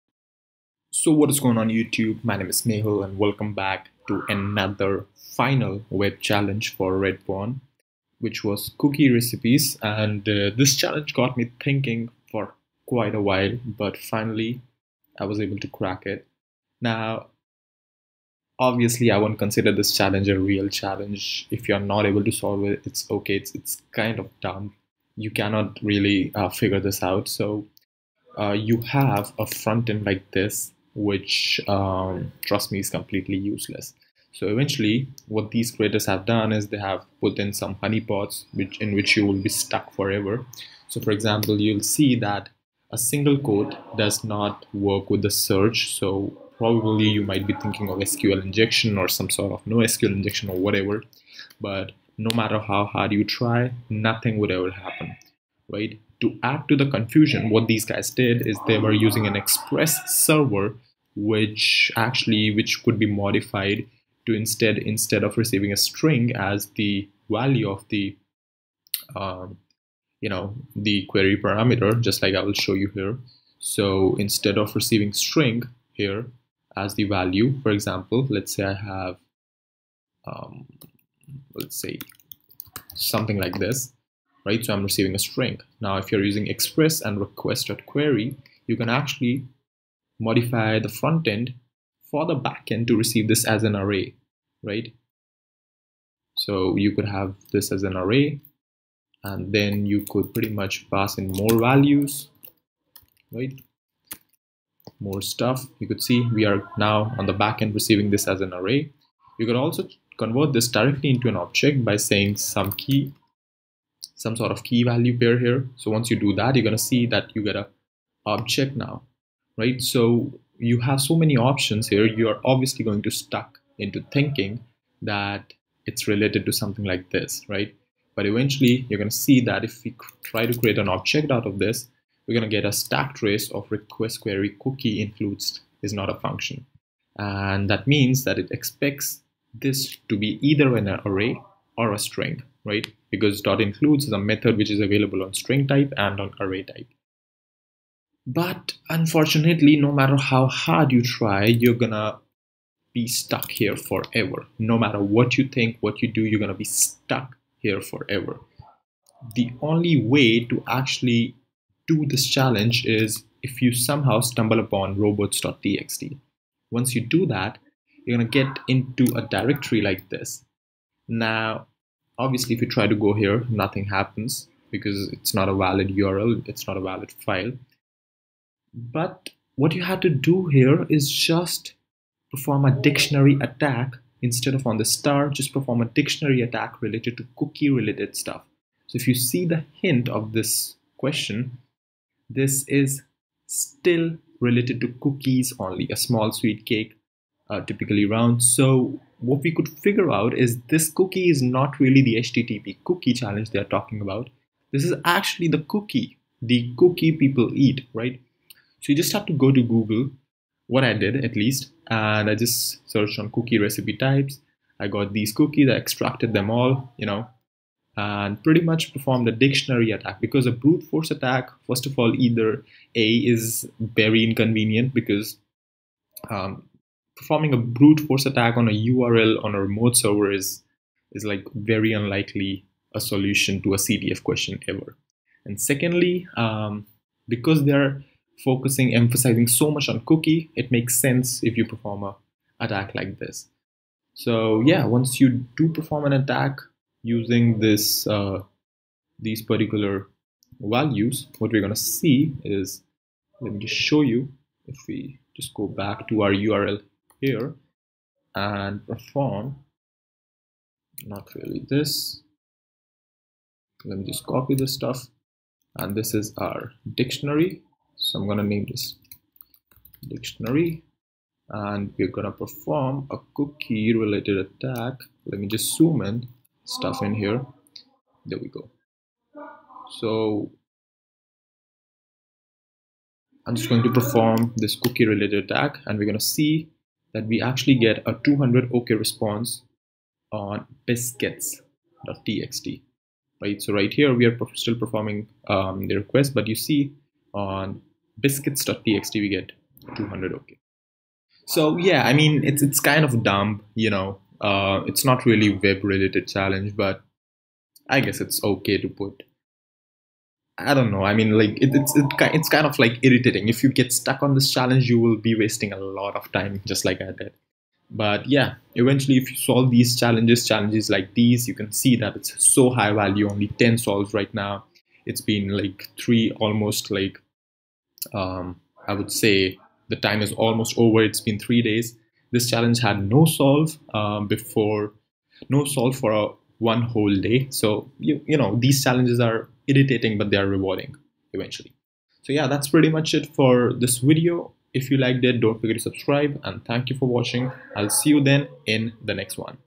Cookie Recipes - RedPwn CTF 2019
This is a question in RedPwn CTF 2019 Web section. Let's see how to approach and solve a question like this.